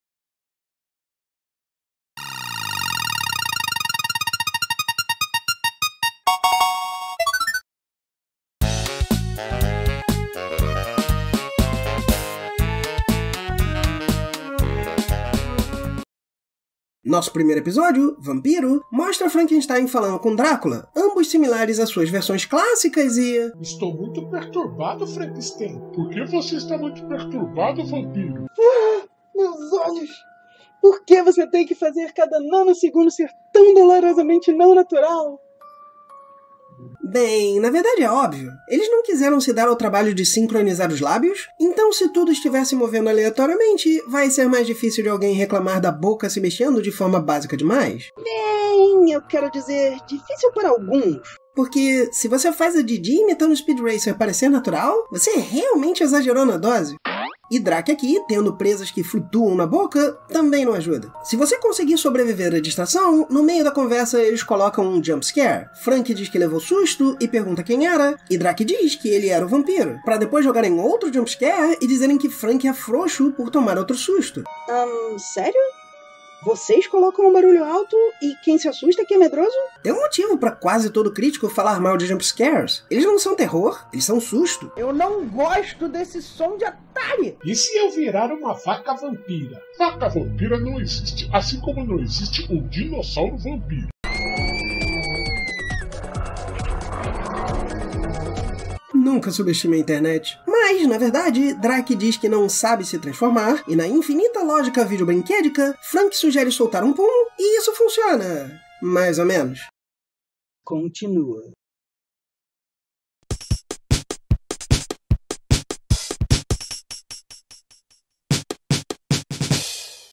Historinhas de Terror: Vampiro | Filmes a Fundo #29.2
Primeira parte de uma série esquecida da Video Brinquedo.